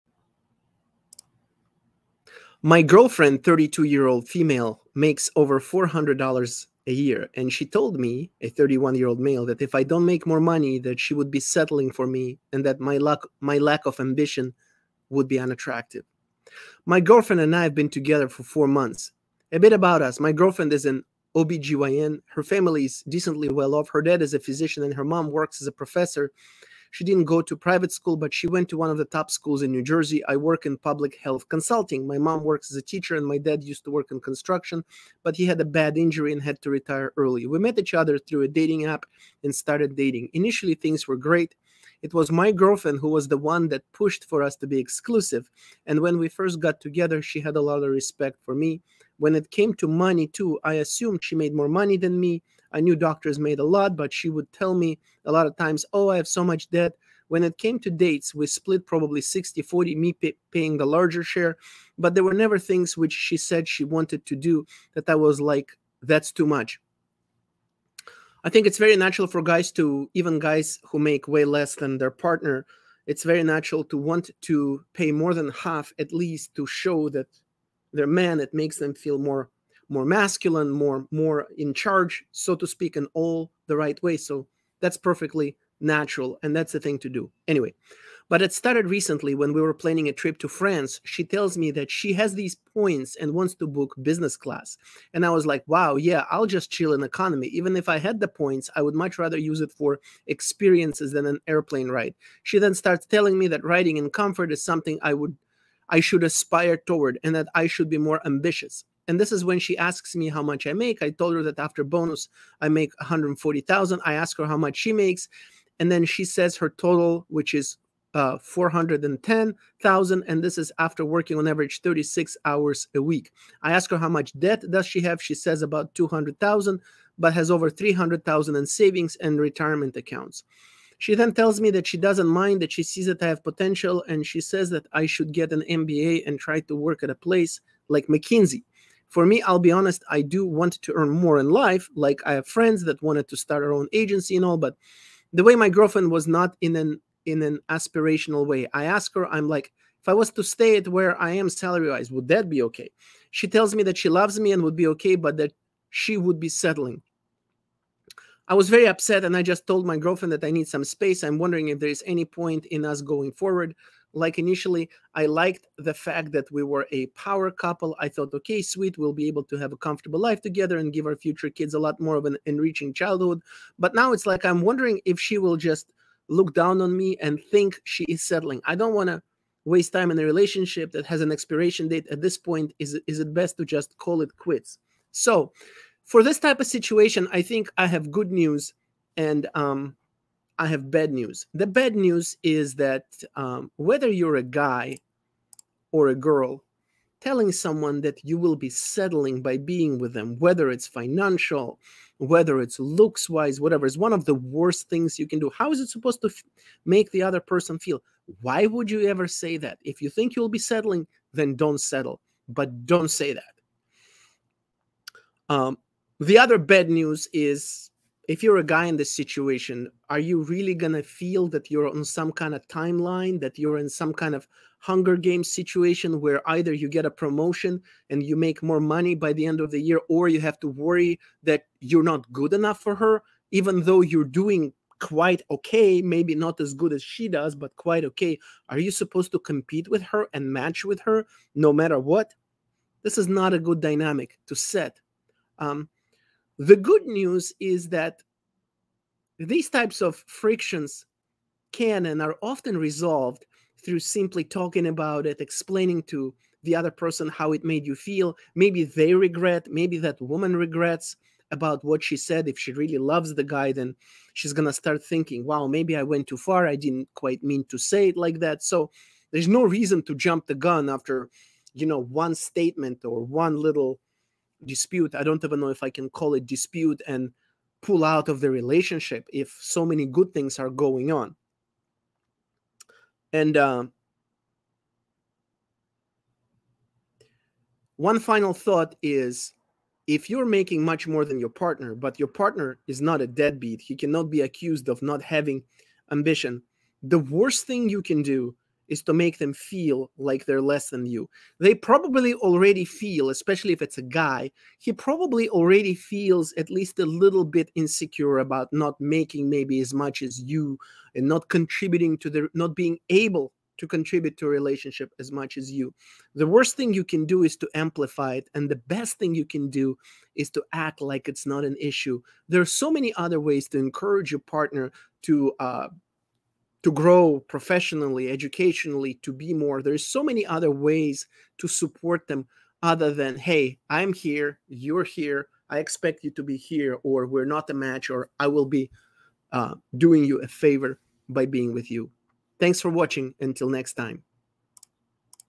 "Settling" For Dating A Guy Who Makes Less Than You?
One of the worst things a woman can do, especially early on in a relationship, is making the guy feel like he is less than her.